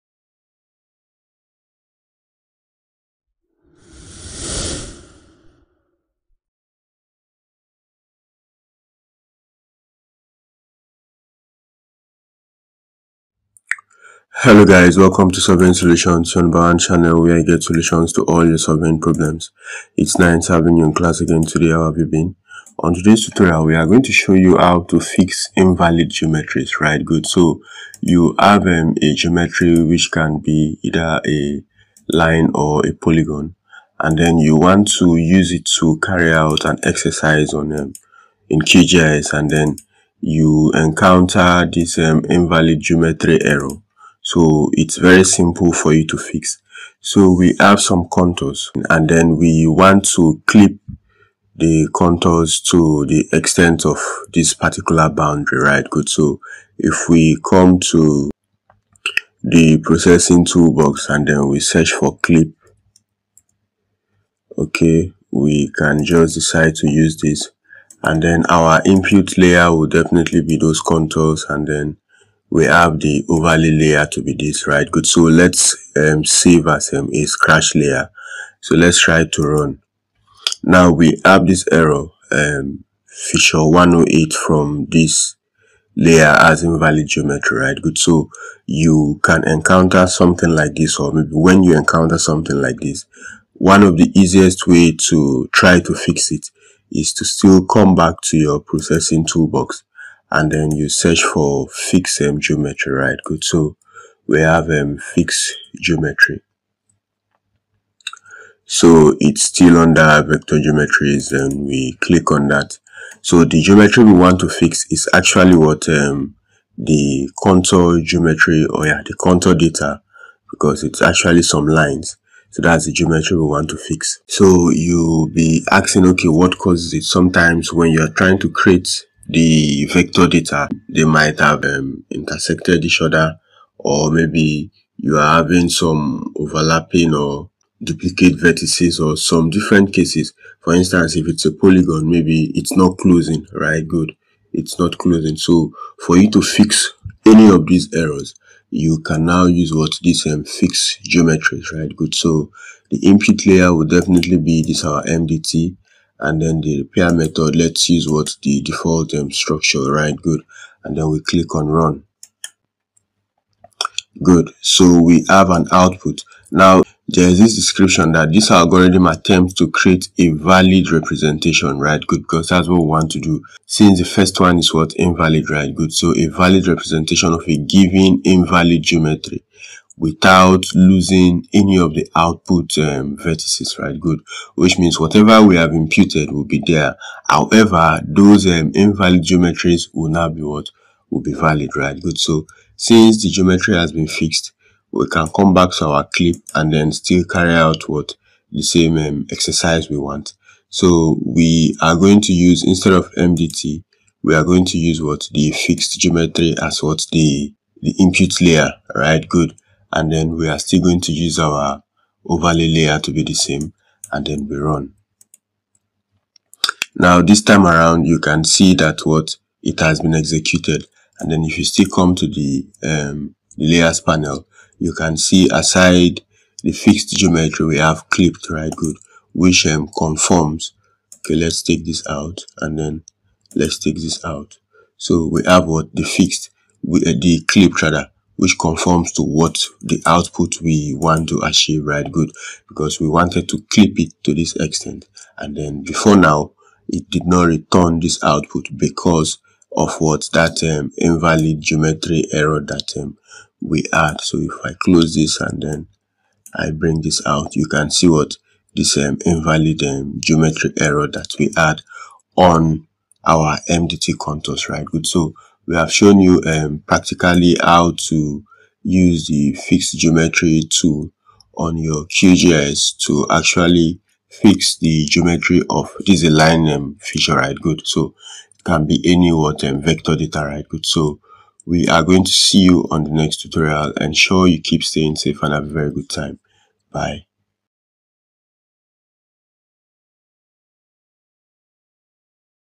Fix Invalid Geometry QGIS | How to Fix Invalid Geometry in QGIS | Handling Invalid Geometries
In QGIS, "Fix Geometries" is a tool used to correct invalid geometries in vector layers. Invalid geometries can cause issues during spatial analysis or when using tools that require valid geometries.
To fix geometries in QGIS, follow these steps:
1. Open QGIS and load the layer that contains invalid geometries.
2. Right-click on the layer in the Layers Panel and select Properties.
3. Go to the Information tab and check if the geometries are valid or invalid.
4. If there are invalid geometries, close the properties window and proceed.
5. From the top menu, go to Vector go to Geometry Tools go to Fix Geometries.
6. In the dialog box that opens, select the input layer (the one with invalid geometries).
7. Choose an output destination (either as a new file or temporary layer).
8. Click Run to fix the geometries.
After this process, you should have a new layer with valid geometries that can be used without issues in your analysis.